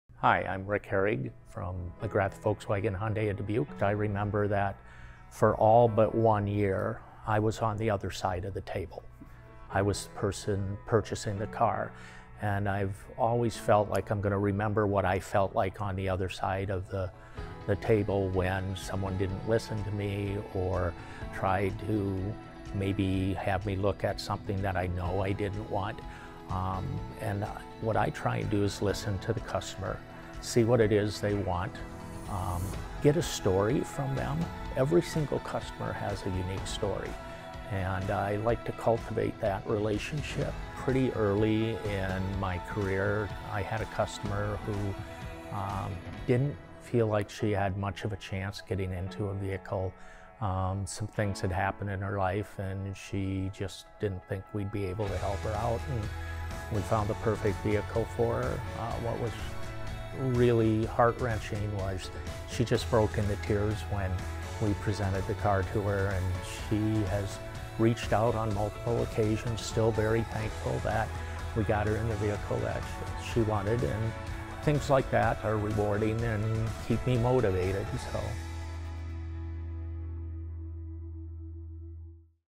We Listen To You! | McGrath Volkswagen Hyundai of Dubuque
Rick is the man! 🎊

Rick is a sales rep here, and has been knocking the socks off every customer who walks in the door since day one. He's been on the other side of the table all but one year of his life, so he has a lot to say about being the one behind it! ✅